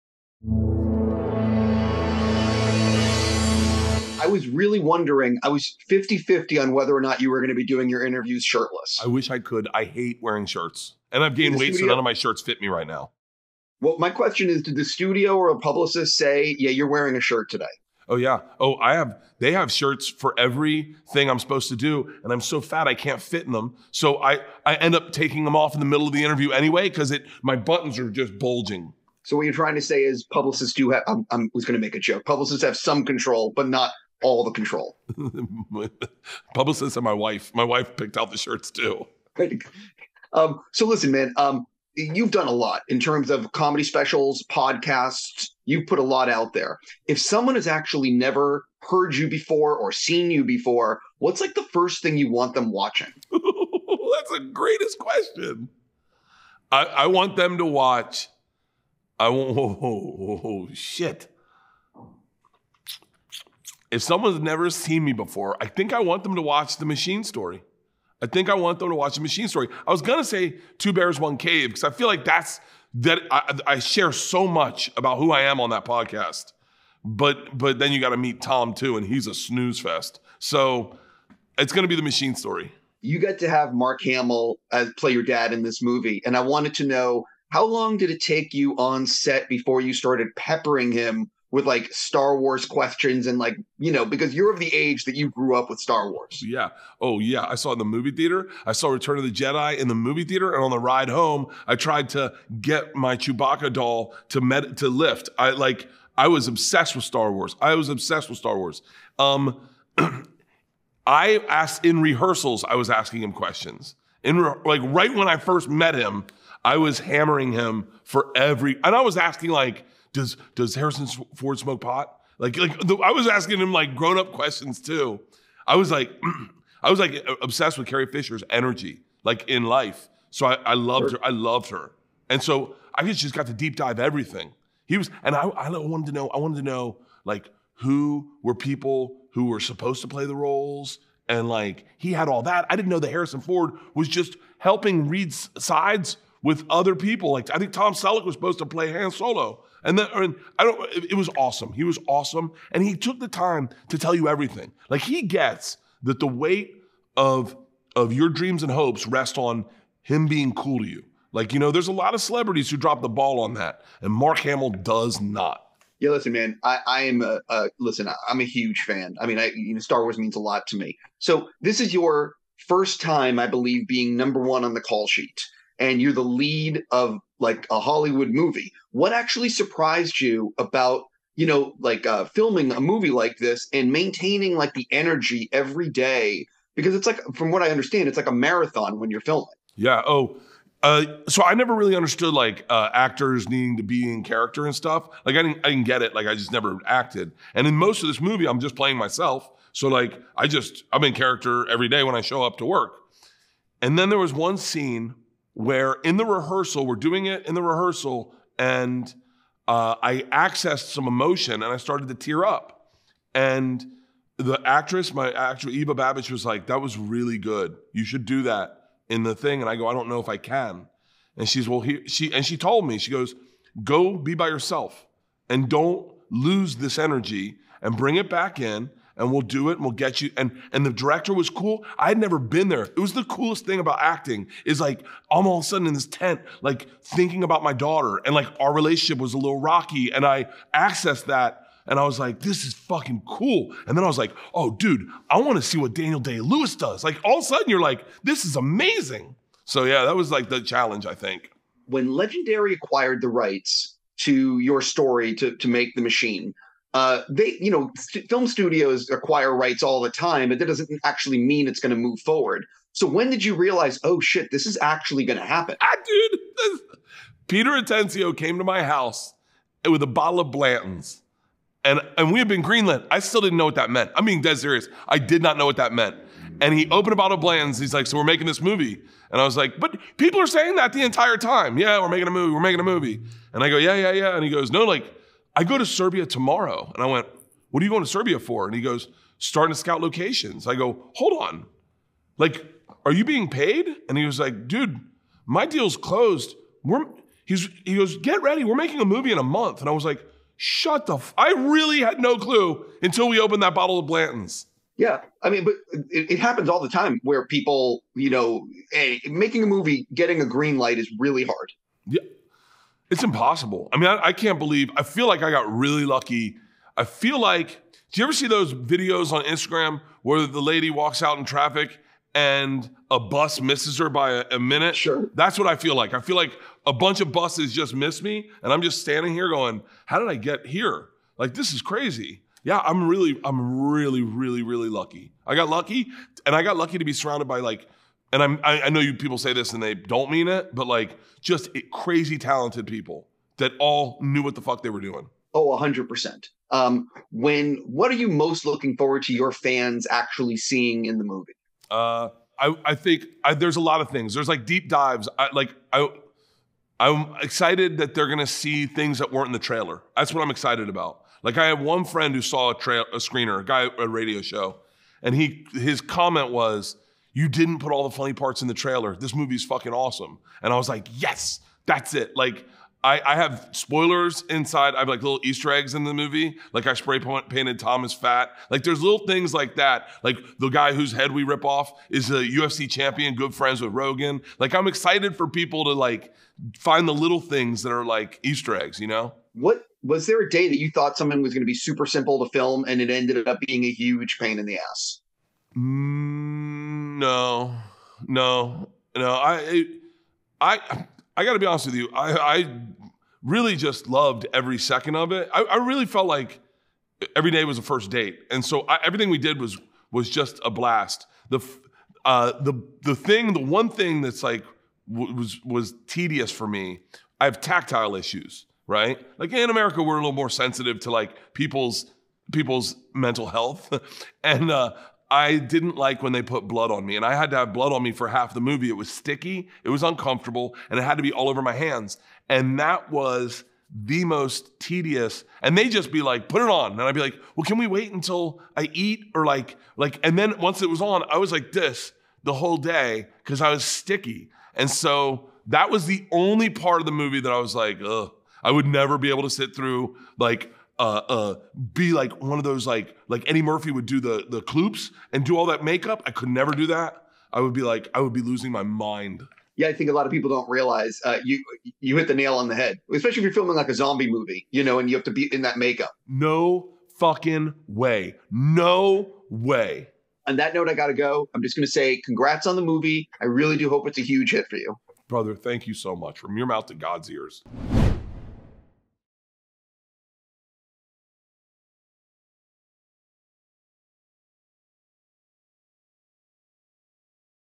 Bert Kreischer Interview: The Machine & Why He Wasn’t Allowed to Do Interviews Shirtless
In a tale of human connection that defies language and cultural barriers, comedian and actor Bert Kreischer makes his theatrical movie debut in The Machine, in theaters May 26. Based on the fantastical stand-up story Kreischer told of his college days attending Florida State University, and directed by Peter Atencio (Keanu), the action comedy takes place 23 years later, when Kreischer (who plays himself) and his father, played by Mark Hamill, must face the consequences of his actions. Collider’s Steve Weintraub spoke with Kreischer about his first-ever leading role in a feature film, and what it was like to lead the charge in a Hollywood movie alongside a Hollywood legend.

The Machine hops between present day and the year 1999, when Bert Kreischer went on a college trip to Russia and befriended a group of Russian mobsters. Two decades later, Bert is a family man, and long gone are the days of “The Machine.” Now, rather than robbing trains with the Russian mafia while drunk on vodka, he’s constantly at odds with his father (Hamill), who doesn’t find his antics entertaining. As fate would have it, both he and his dad are kidnapped by a woman who claims “The Machine” was involved in a robbery that tangles them up with a sociopathic family, landing them back in Russia. The Machine also stars Jimmy Tatro as young Bert, Iva Babic, Martyn Ford, and Stephanie Kurtzuba.

Before taking this wild train ride, check out Kreischer’s interview with Weintraub in the video and transcript below, and find out what he learned in his Star Wars “deep-dive” with Hamill, how he tapped into his own Daniel Day-Lewis on set, and what about film production he found “the most tedious.” Kreischer also shares what he’s most excited for his fans to see and the hidden Easter eggs planted in the movie for audiences to catch.